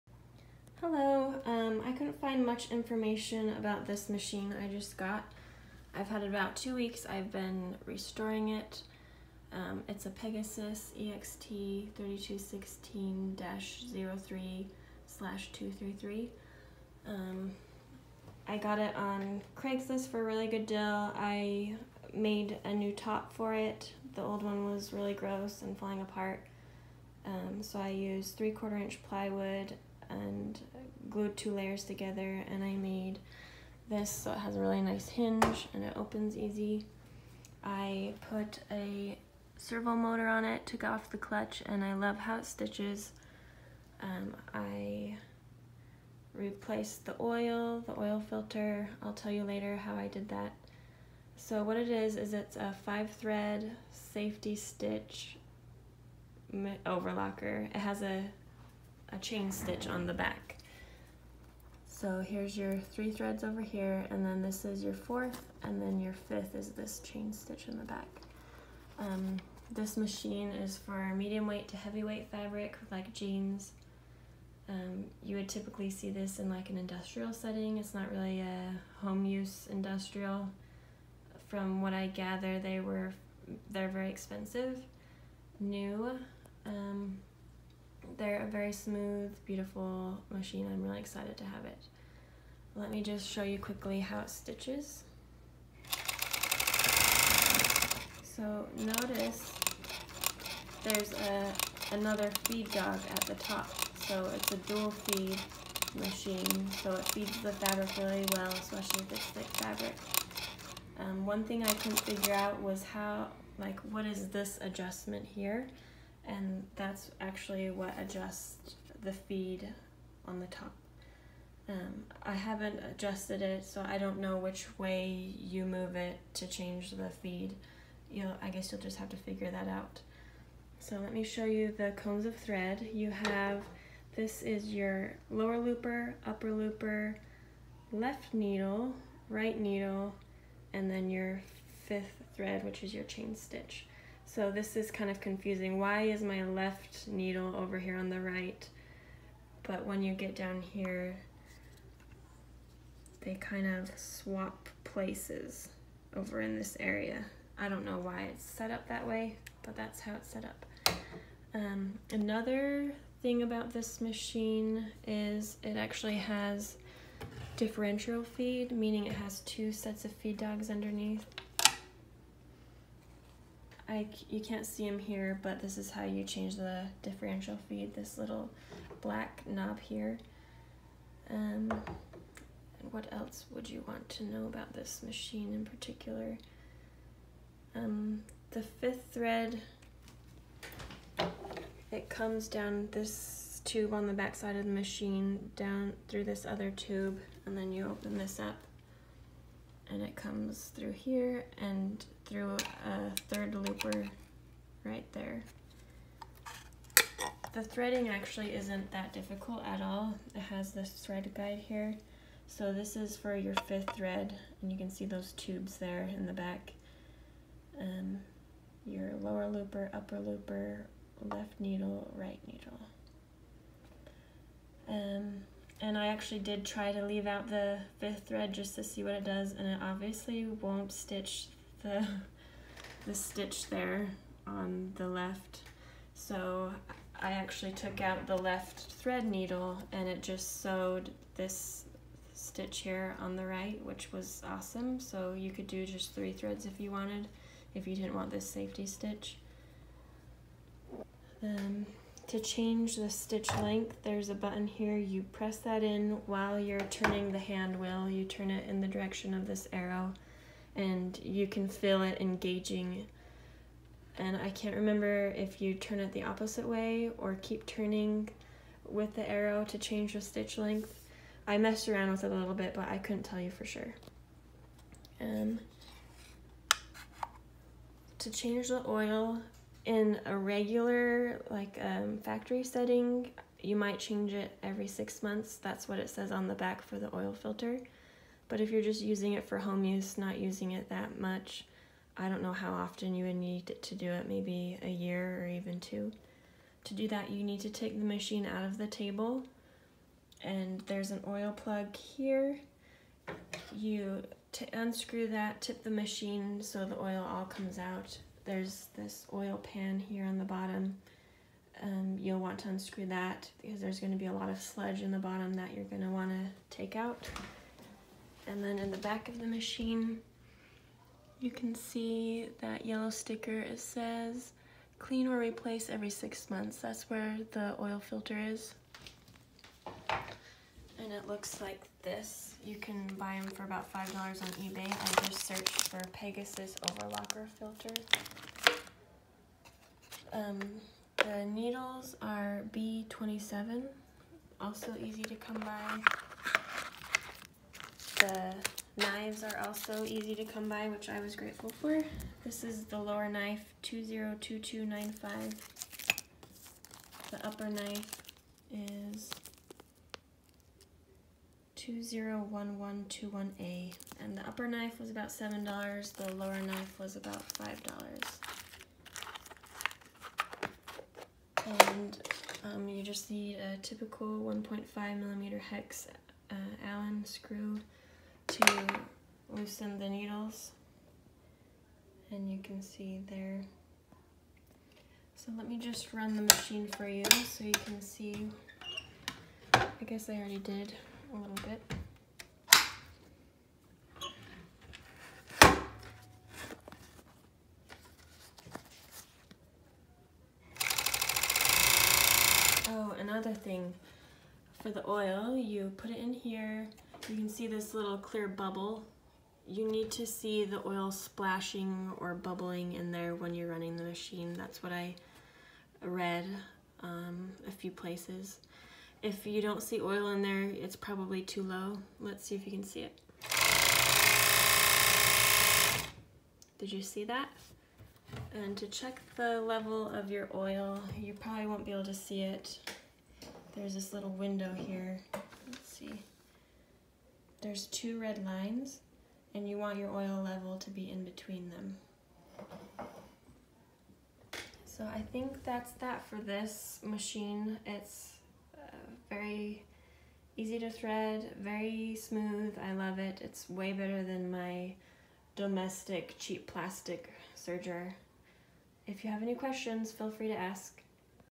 Pegasus EXT 3216-03/233 Industrial Overlocker
Beautiful machine but not a lot of info on it out there, hoping this video helps!

It’s a 5 thread safety / chain stitch overlocker that has dual feed (feed dog on bottom AND top) as well as differential feed (the two lower feed dogs can move at different rates).

Have fun sewing!